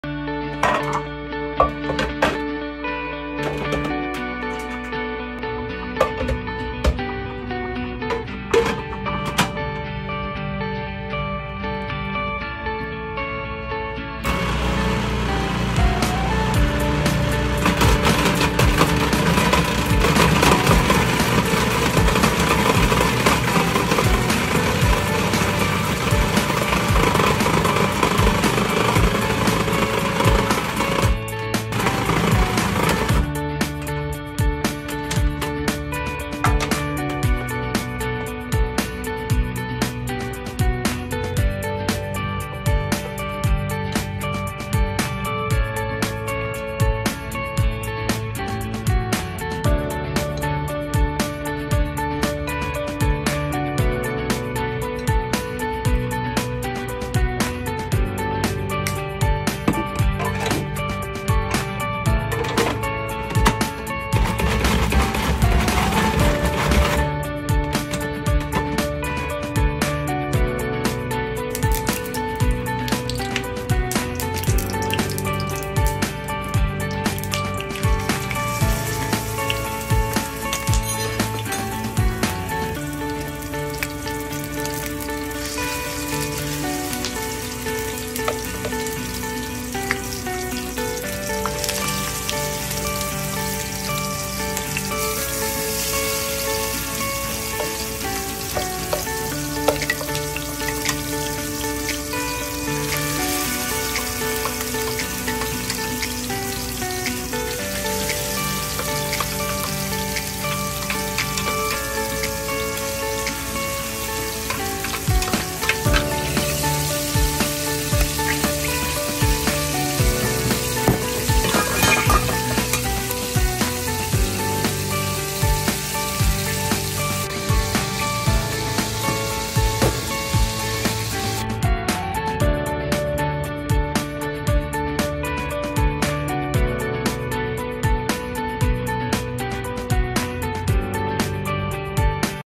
フードプロセッサーがあれば簡単❣️イカ団子
フードプロセッサーでイカ団子

イカ2杯
ネギ1/2
生姜大2
片栗粉大1
醤油小さじ2

大葉と大根おろし

フードプロセッサー
メタルで回す

エサンテ油をいれ
フライモード180度で温める

しっかりあたたまったら
スプーンで落とし入れる

蓋をしてスチームが出るくらい熱を入れたら
ヘラでびっくり返す

揚げ焼きにする

大葉、大おろしを乗せて
醤油をかけていただく

今からの季節
お鍋に入れても良いね

時短レシピ#簡単レシピ